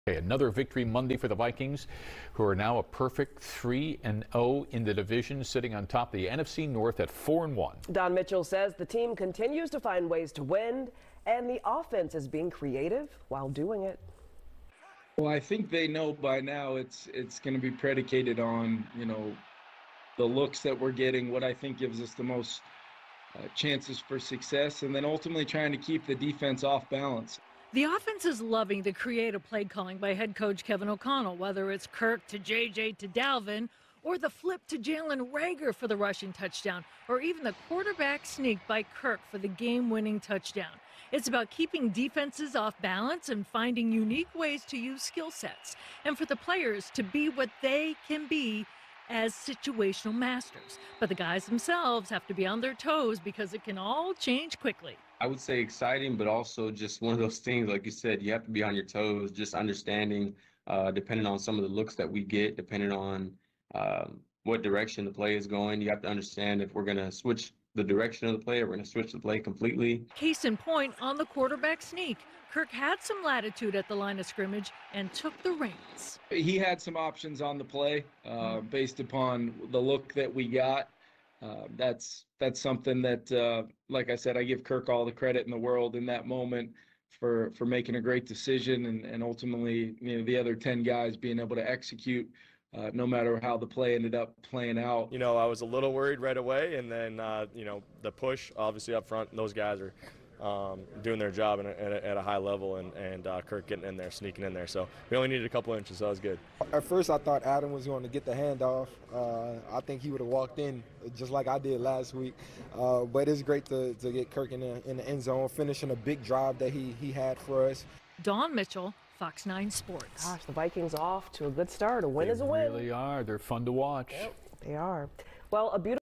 Vikings learn lessons after rallying for 29-22 win over Bears
The Minnesota Vikings are 4-1 and 3-0 in the NFC North after rallying for a 29-22 win over the Bears on Sunday.